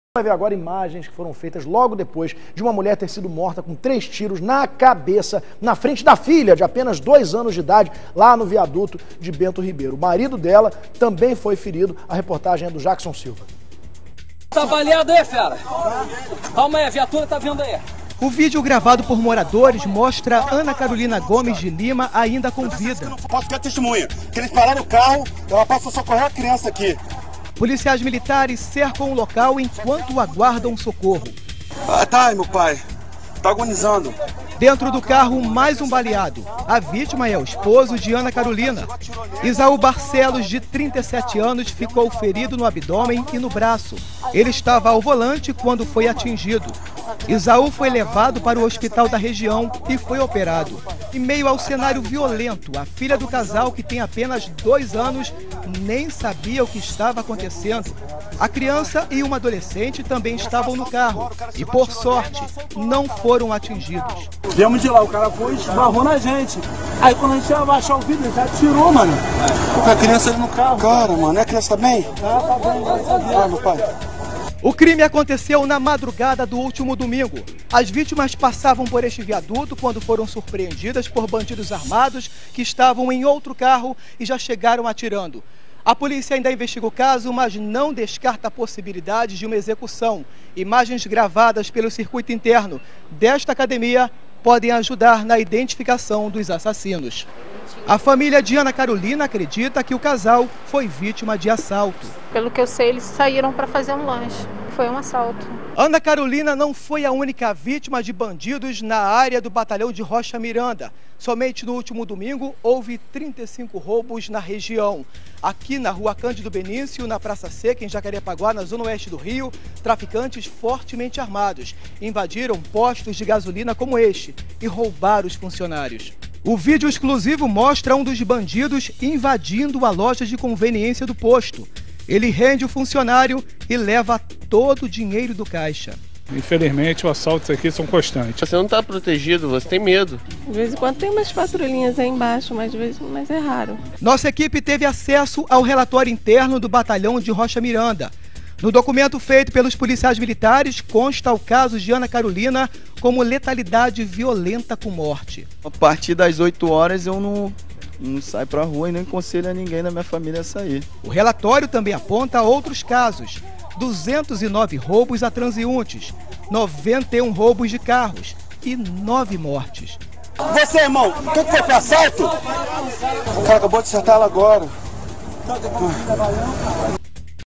Novas informações sobre caso de assassinato em Bento Ribeiro
Você vai ver agora imagens feitas logo depois de uma mulher ser morta com três tiros na cabeça, na frente da filha de apenas dois anos, no viaduto de Bento Ribeiro. O marido dela também foi ferido.